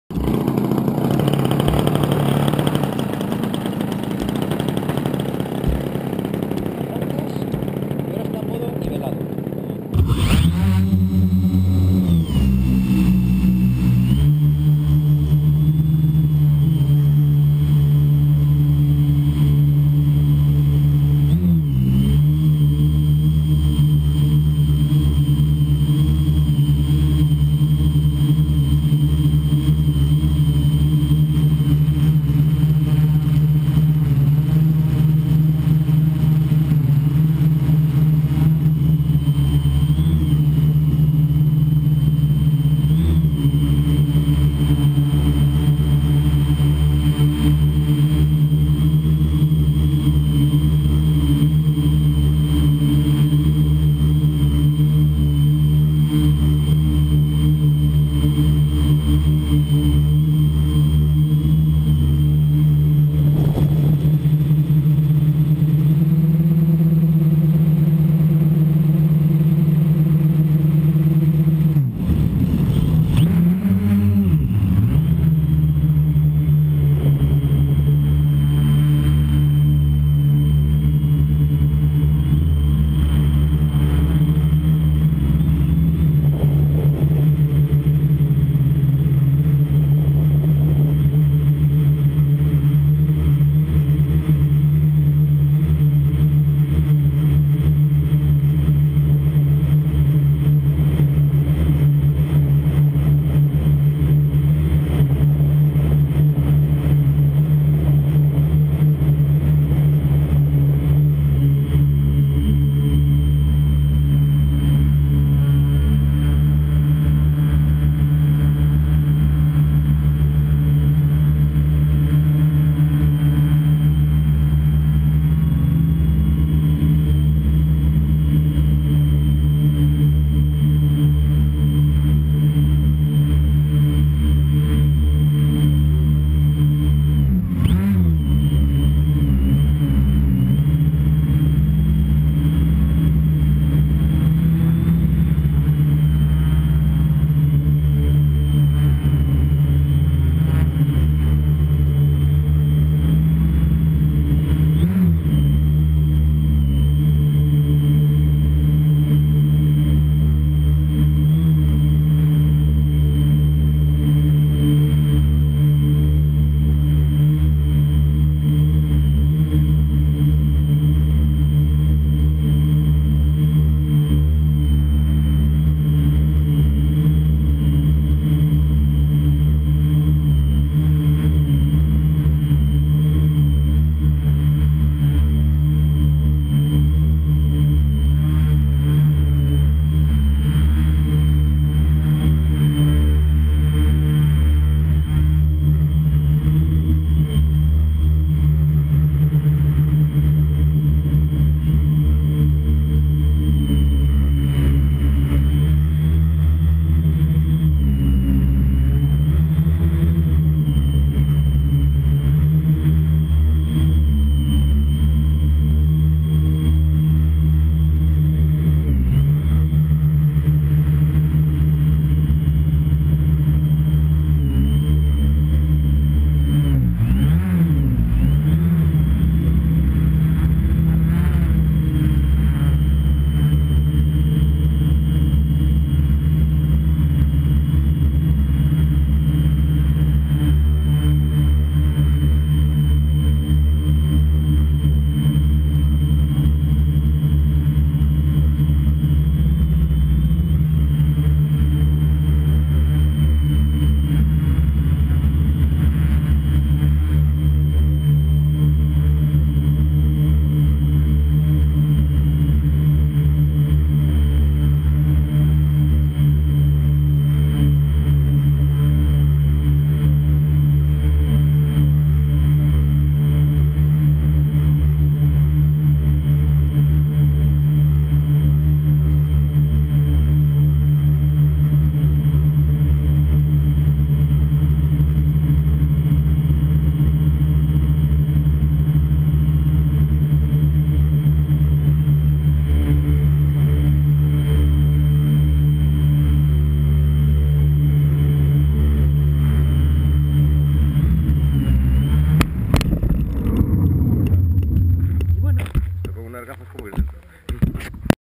Club RC Ourense
Vídeo grabado en las instalaciones del Club RC Ourense con un Gaui 500X-S con Gu-Ins